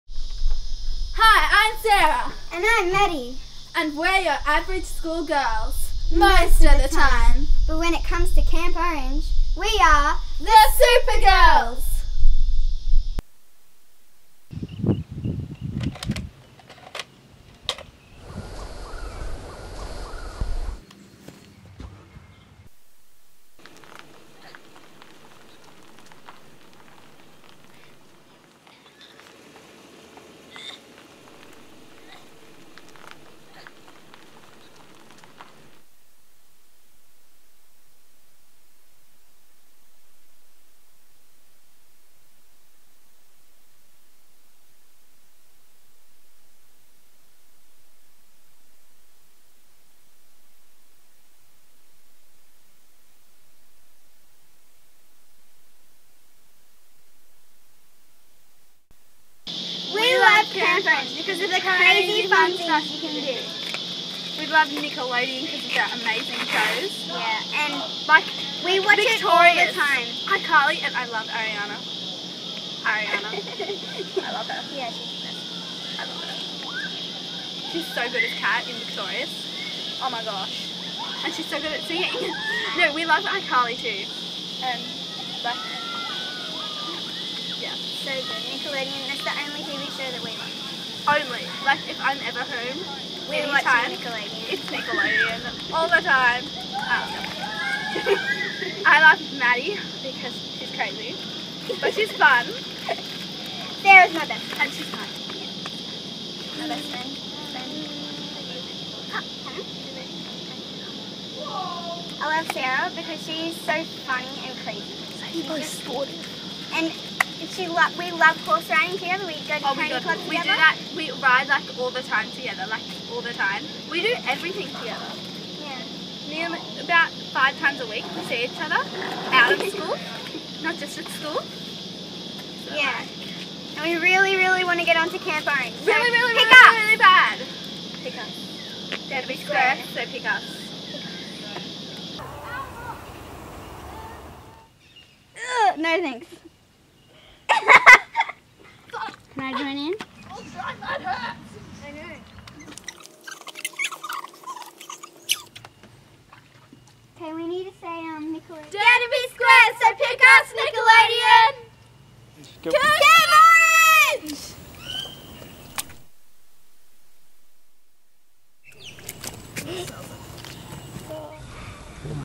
Sarah & Maddy audition for Camp Orange 2013
Sarah & Maddy (The Super Girls) in Sydney, Australia put together this audition for Camp Orange 2013. It is their first and last attempt as both will be too old next year.

Update: Well they didn't make it to Camp Orange, but they did make it to one of the ads on Nickelodeon in May where they were shown saying "Dare to be square. Pick us Nickelodeon". So we at least know they looked at the audition video.

(CampOrange-Ipad.mp4)